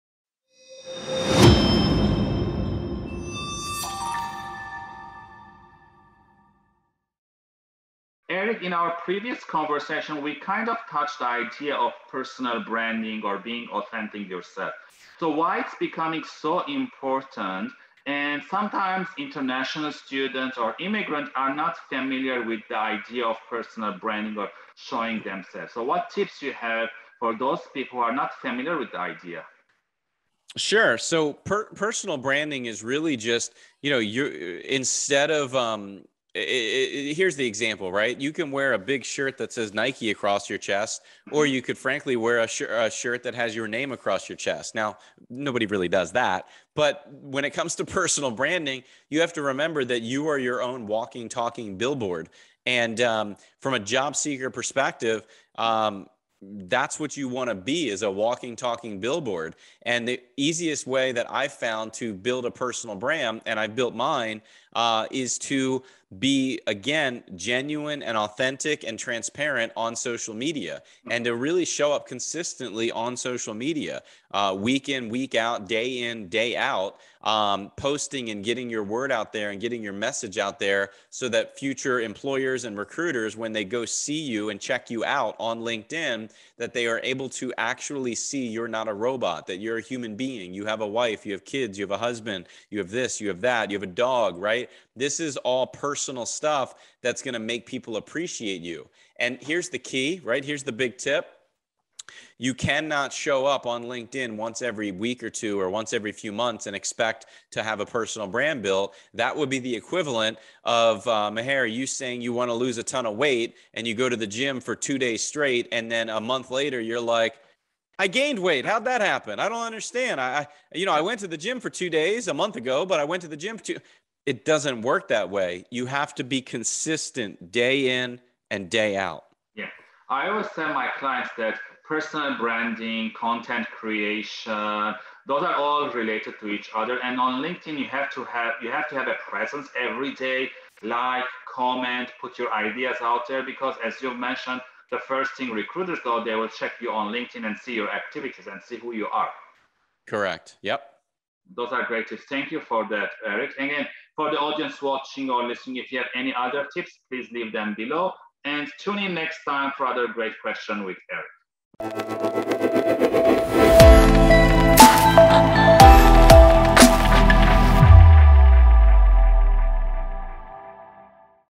How a Personal Brand Can Help Your Job Hunt
It is important these days to know who you are and what are your values.
You need to be transparent, authentic and genuine on social media.
You need to be consistent in publishing original content.